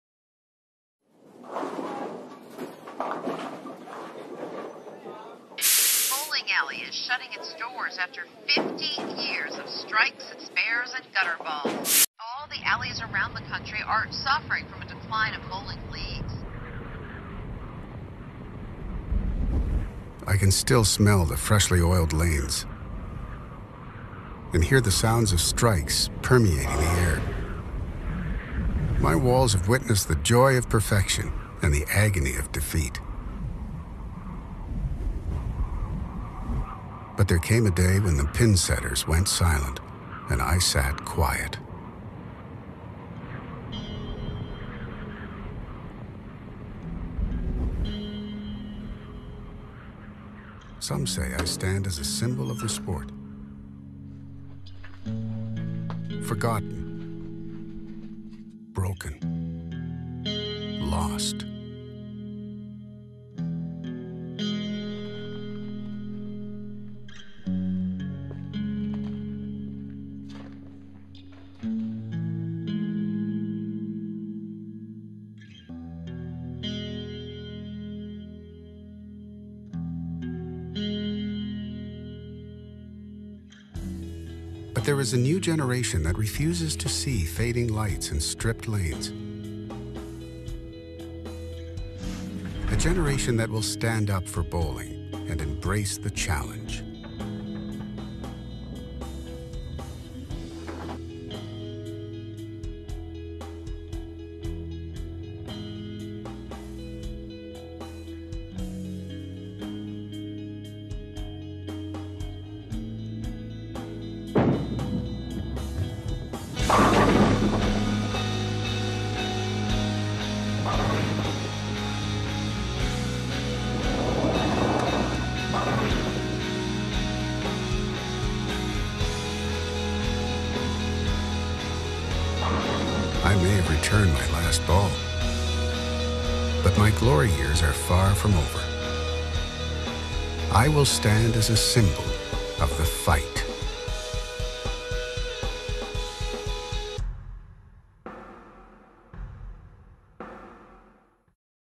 Storm FIGHT
2016.2月発売のＳＴＯＲＭ「ファイト」のイメージビデオです。
マーシャル・ケント（Marshall Kent）